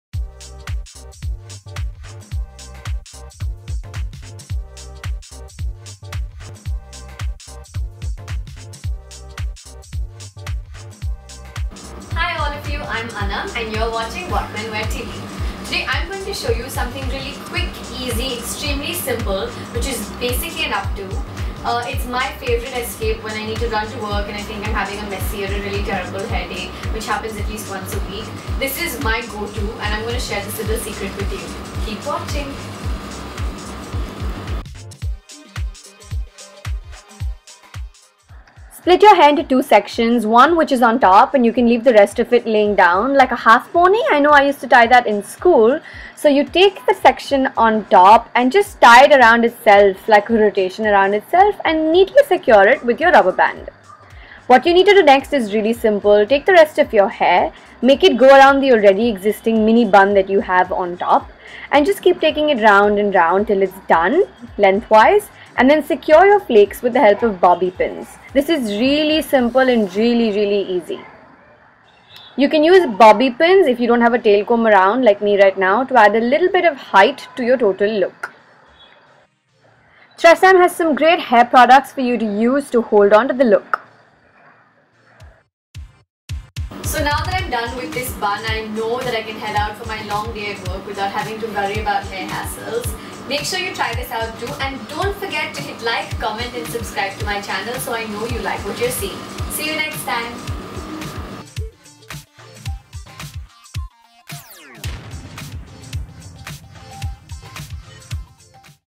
2 Minute Up Do | Perfect For Work | Everyday Look
I shot this video tutorial a little before I had my hair cut. Personally,I believe that it's perfect for a quick run to work or to hide a hideous hair day. Take a look and let me know what you think of it!